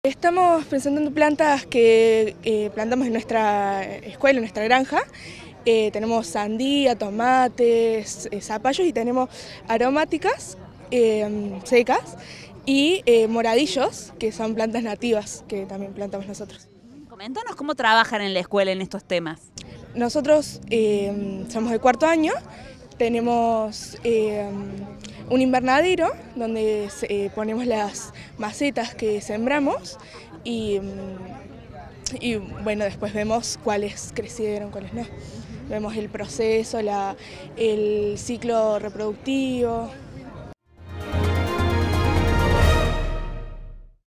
CON LA PARTICIPACIÓN DE MÁS DE 30 EXPOSITORES SE REALIZA UNA NUEVA EDICIÓN DE LA FERICAMBIO
PAULA – ESTUDIANTE ESCUELA DE AGRONOMÍA